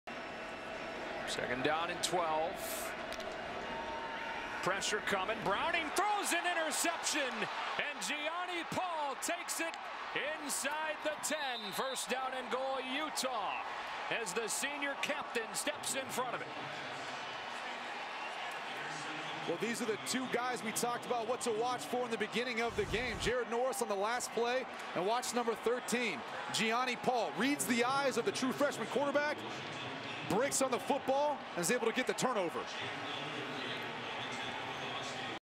2015 Utah vs Washington - Gionni Paul Interception
Jake Browning pass intercepted Gionni Paul return for 27 yds to the Wash 8